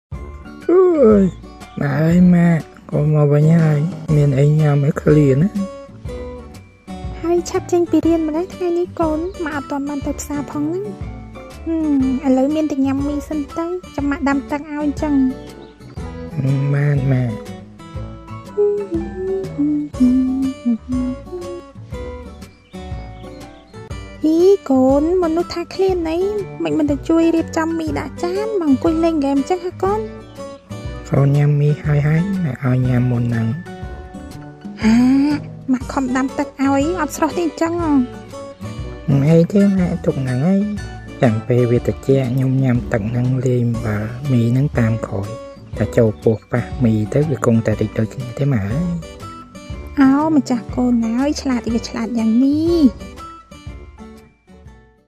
រឿងខ្លីៗ "វិវឌ្ឍនកាលនៃមីស្រុះ"
កូនមកដល់ផ្ទះឃ្លានខ្លាំងតែម្ដាយអត់មានម្ហូបមានតែឆុងមីអោយញ៉ាំ ឃ្លានក៏ឃ្លាន ចង់លែងហ្គេមក៏ចង់។